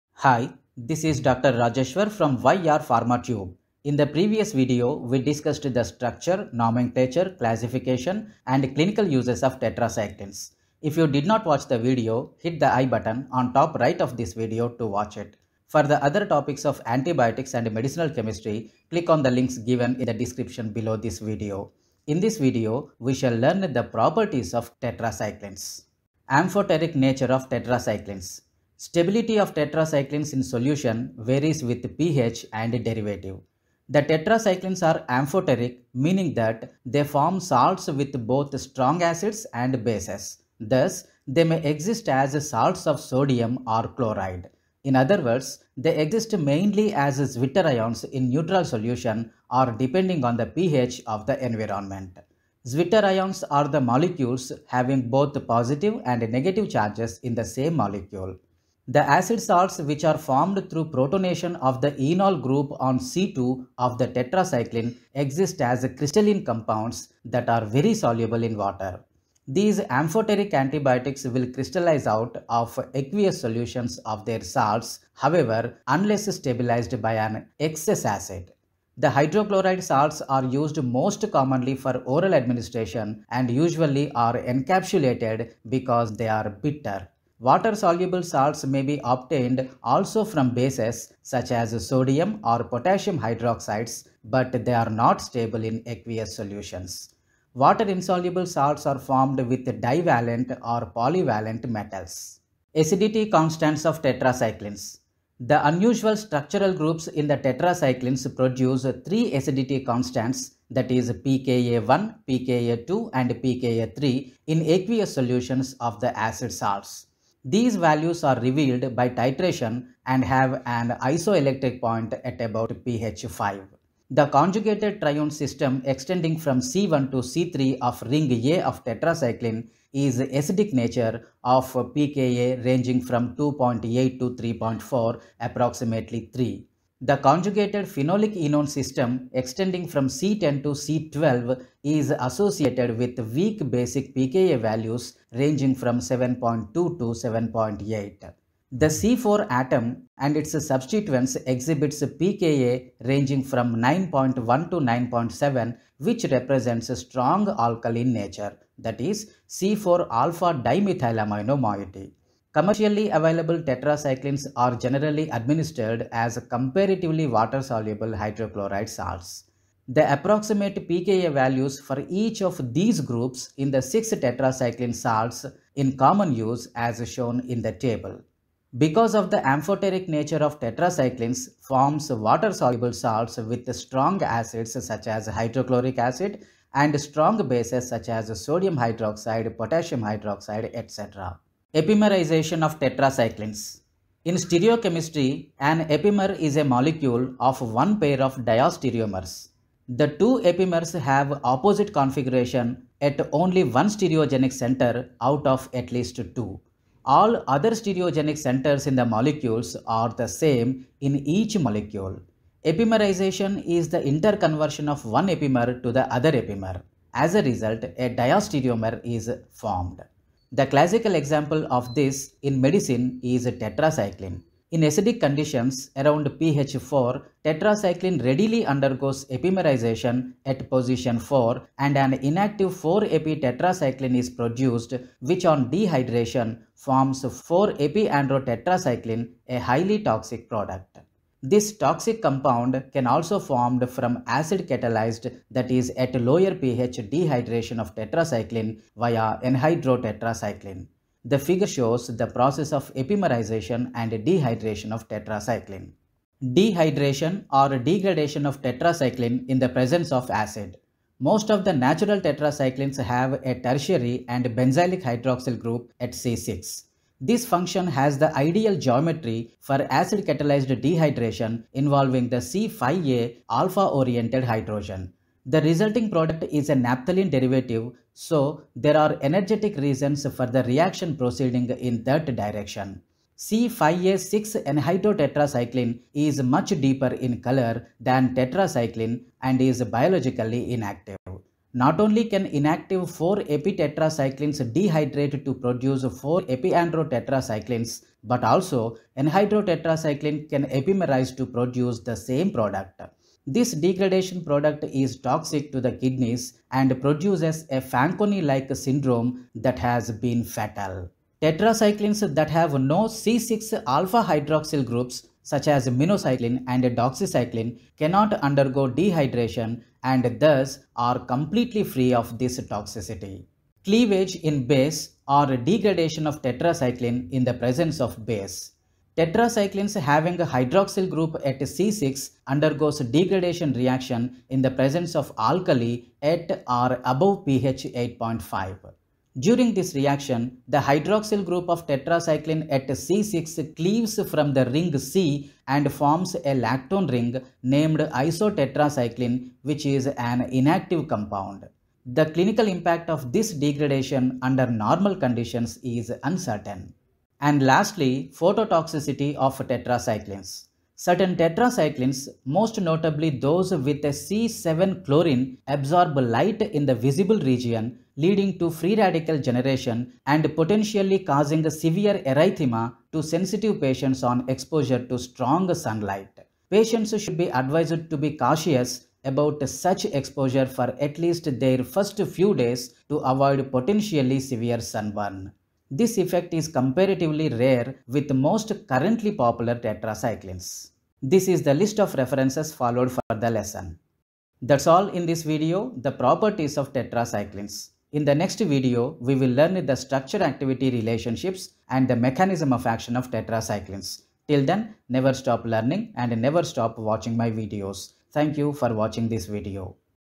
Antibiotics - Tetracyclines: Amphoteric nature | Acidity constants | Epimerization | Degradation
Also watch,